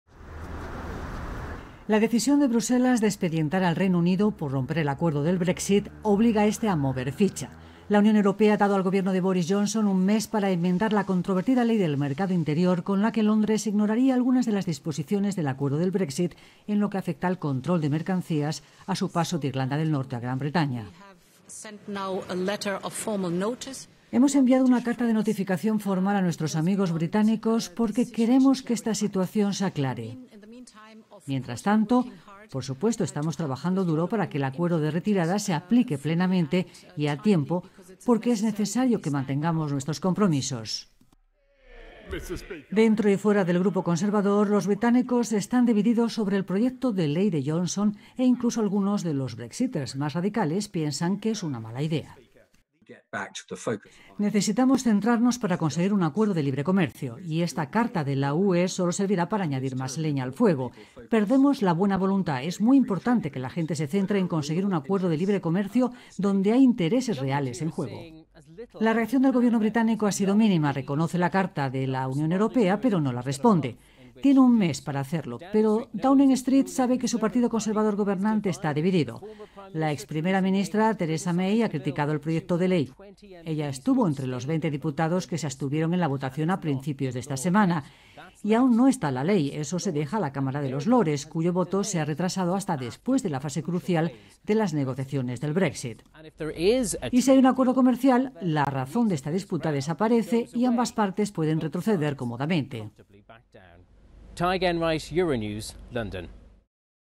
La UE expedienta al Reino Unido por el Brexit
La Comisión Europea  inicia un proceso para denunciar al Gobierno de Boris Johnson por incumplir el acuerdo del Brexit